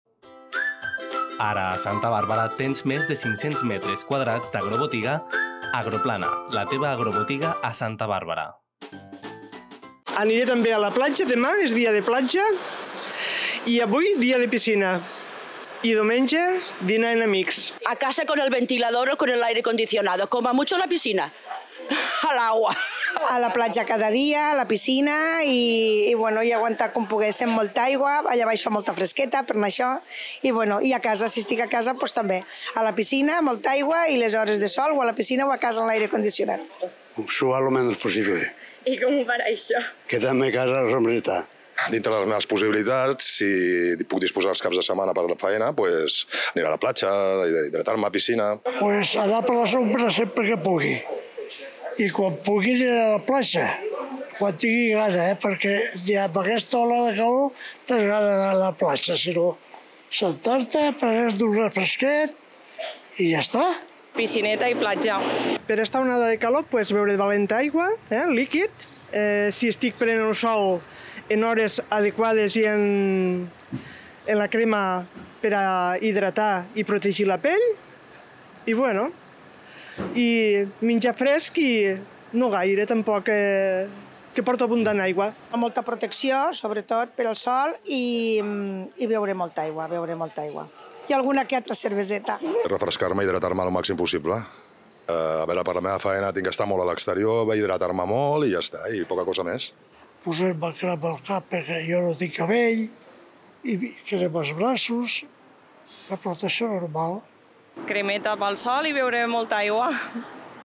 Els ebrencs aposten per la piscina o la platja per combatre l’onada de calor
Arriba un dels caps de setmana més calurosos de l’any. Avui hem sortit al carrer per preguntar quins plans tenen els ebrencs per combatre aquestes altes temperatures. La piscina o la platja són les opcions més populars.

Els ebrencs tenen la lliçó ben apresa pel que fa a les mesures per passar el millor possible aquesta onada de calor. Beure molta aigua, evitar les hores de sol i una alimentació lleugera són les millors pautes.